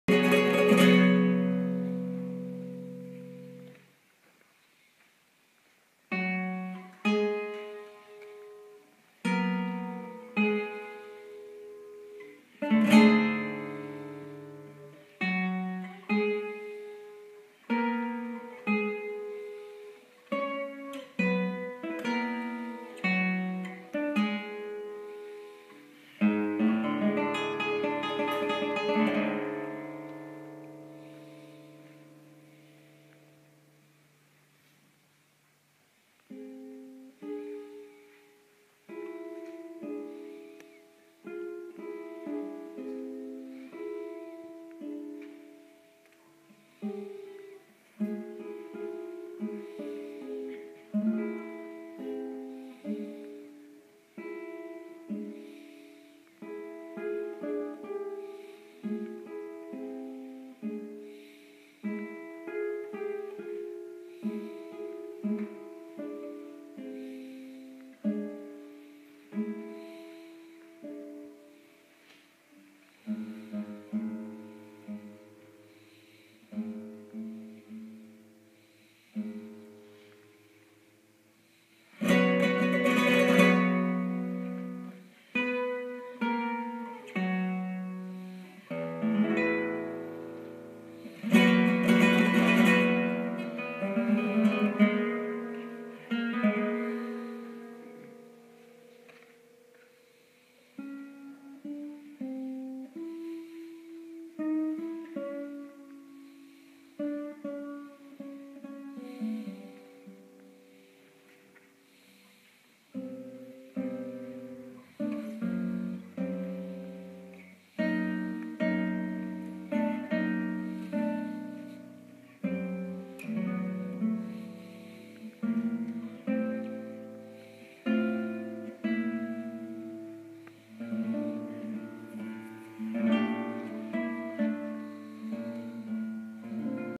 Giulia Ballaré guitarrista italiana en Certamen Andrés Segovia La Herradura 2018
La guitarrista italiana, Giulia Ballaré durante la actuación en la Primera  Fase Eliminatoria  del Certamen Guitarra Clásica Andrés Segovia La Herradura 2018